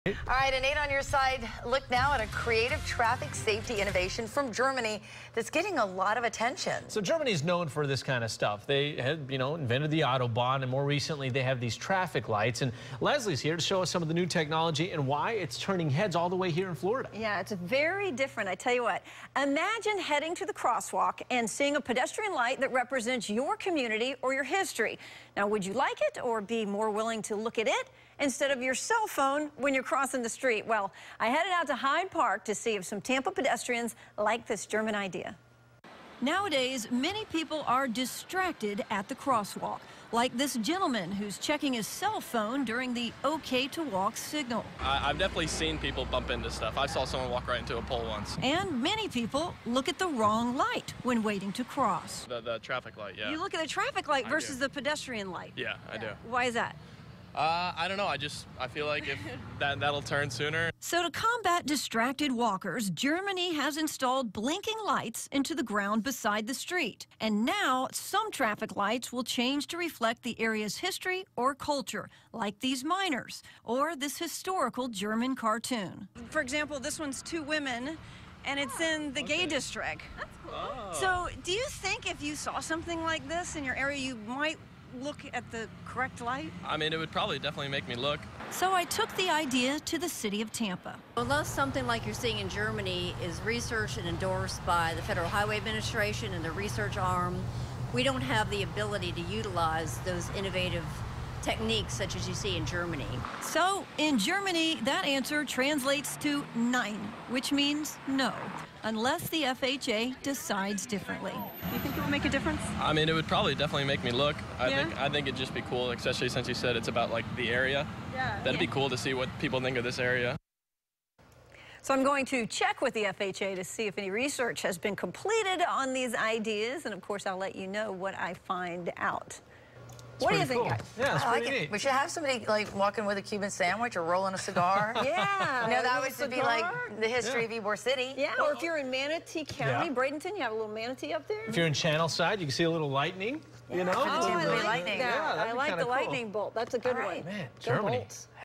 Cultural traffic signals
Germany tries out cultural traffic signals